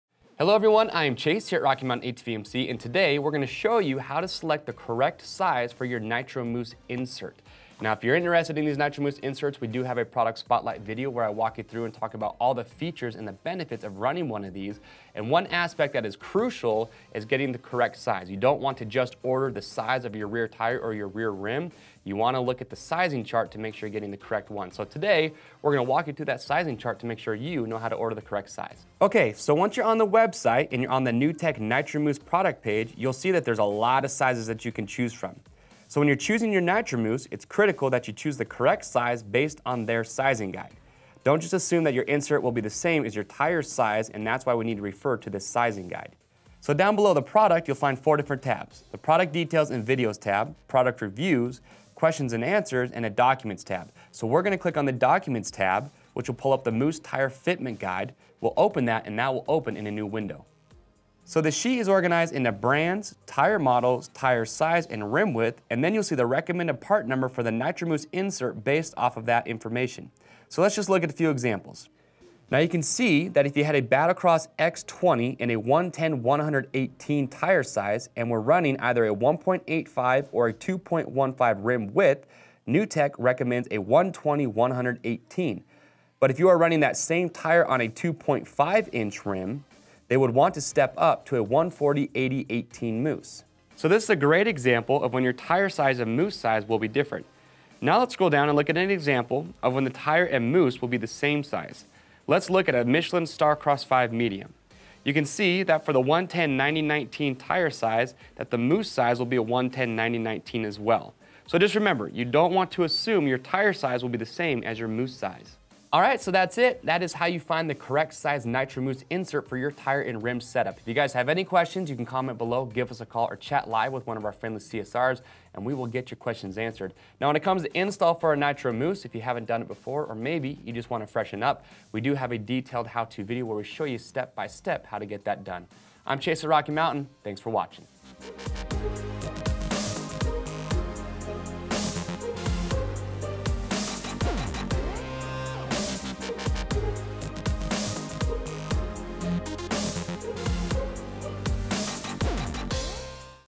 Nuetech Nitro Mousse Foam Motorcycle Tube Sizing Guide
Nuetech Nitro Mousse Foam Motorcycle Tube
100% Flat-Proof – Nitromousse is a solid foam tire insert that completely eliminates the possibility of getting a flat.

Better Feel – Nitromousse’s proprietary nitrogen charged technology provides a more consistent and lively feel that eliminates the "dead" feel of other mousses, which gives Nitromousse a more pneumatic feel.

2X Durability – Nitromousse is more resistant to heat and friction, which gives it a much longer life without compromising performance.

Fitment Chart - Nitromousse provides tire-specific fitment so you get the right mousse for your tire the first time and eliminate the guessing game. Find the correct size mousse for your tire above in the Documents tab next to the Questions & Answers tab or by using Neutech's online application guide here.